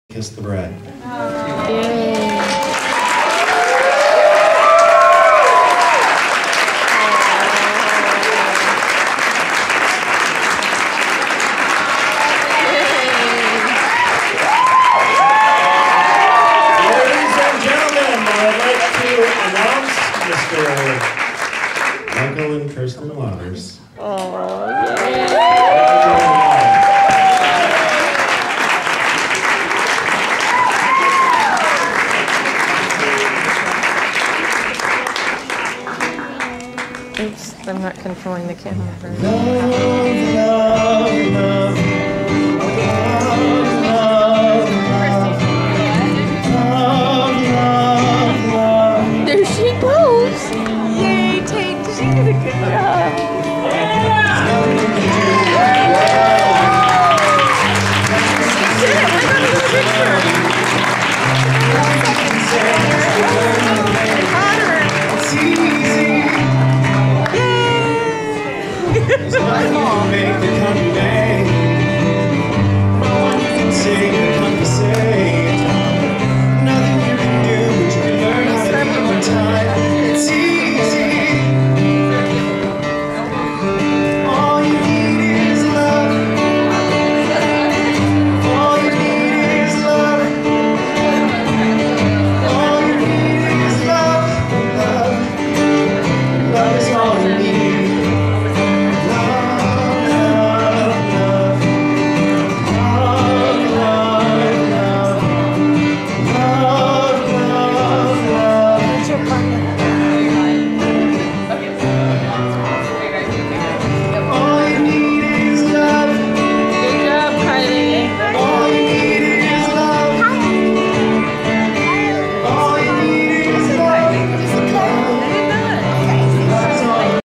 Mike and Kirsty Lauters "First Kiss" OCT.26th, 2013 WEDDING
Mike and Kirsty Lauters first kiss as man and wife at their wedding Oct.26th, 2013